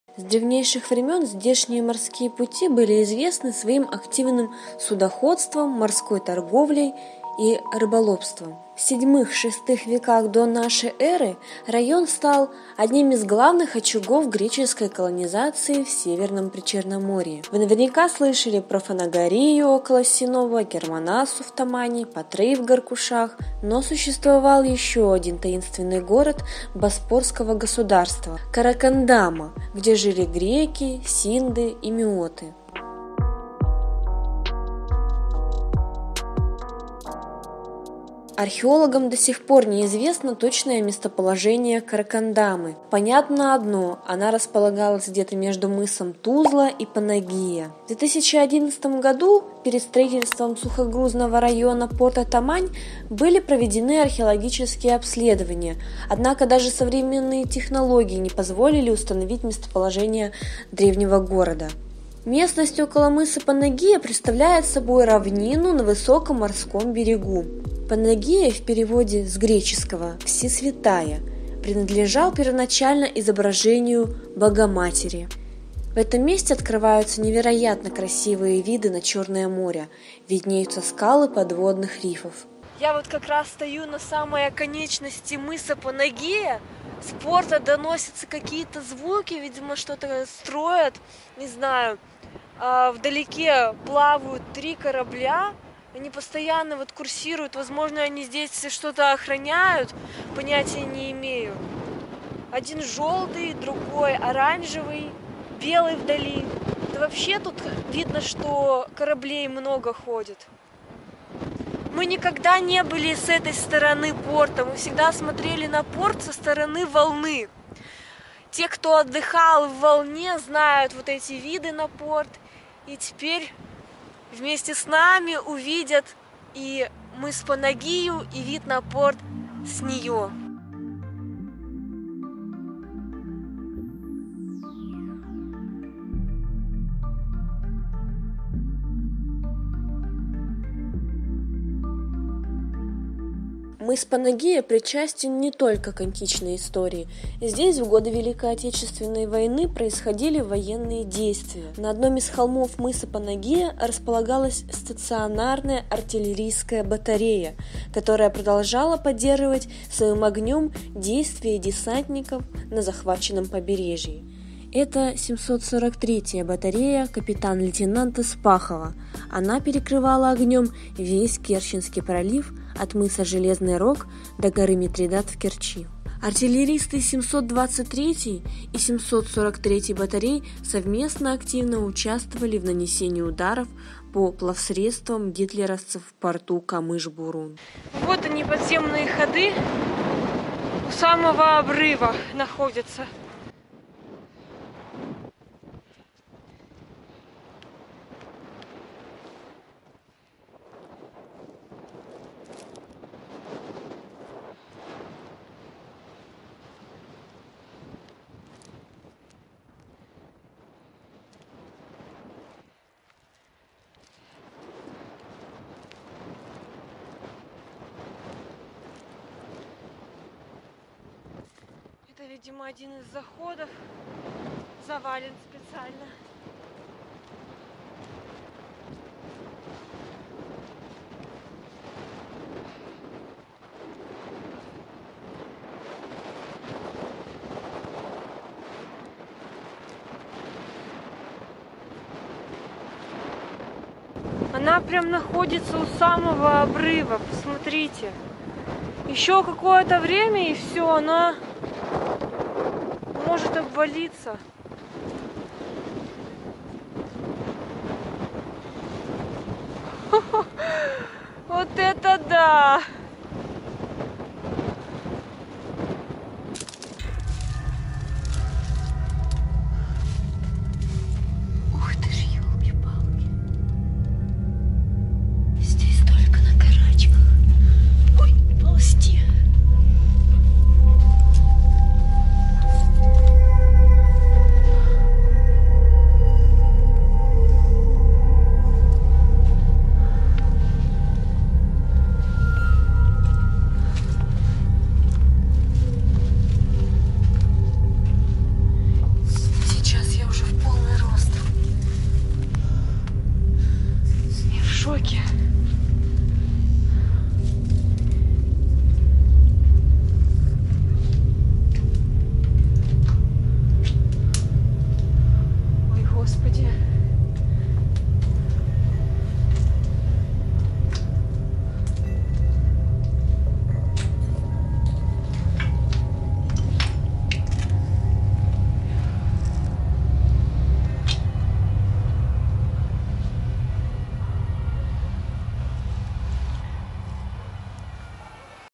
Мыс Панагия, Тамань: артиллерийская батарея 743
Берега Черного моря от Крымского моста на косе Тузле до мыса Панагия у порта Тамань были заселены людьми ещё в древности. В годы ВОВ здесь проходили военные действия на море.

Добровольная помощь в развитии канала:
 Сбербанк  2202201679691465 КСЕНИЯ СЕРГЕЕВНА С.